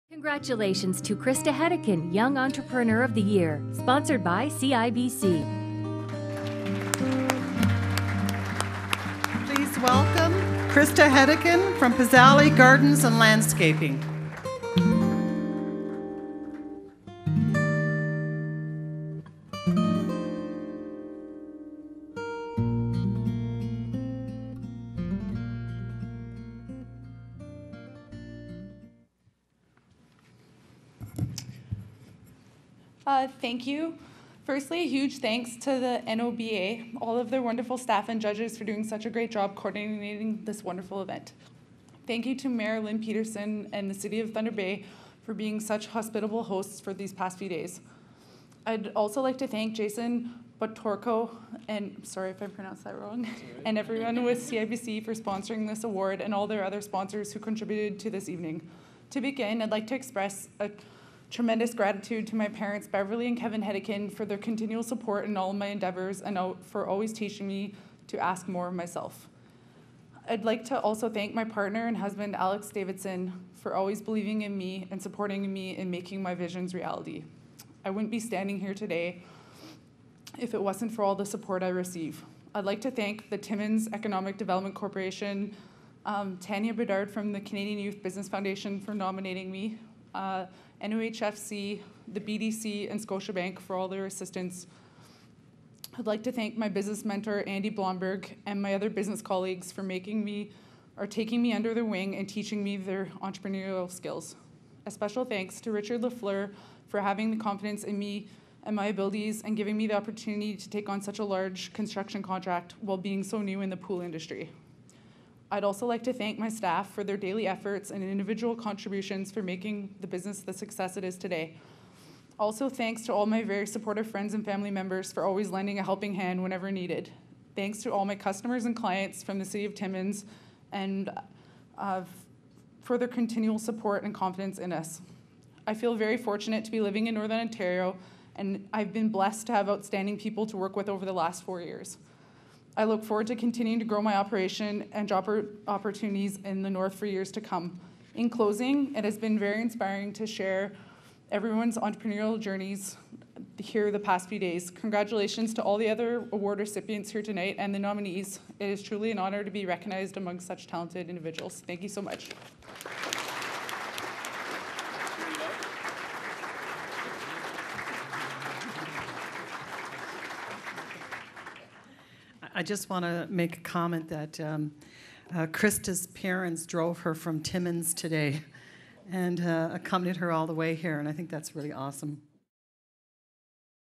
Young Entrepreneur of the Year - Krista Hedican
Krista Hedican acceptance speech during the 2010 Northern Ontario Business Awards (NOBA).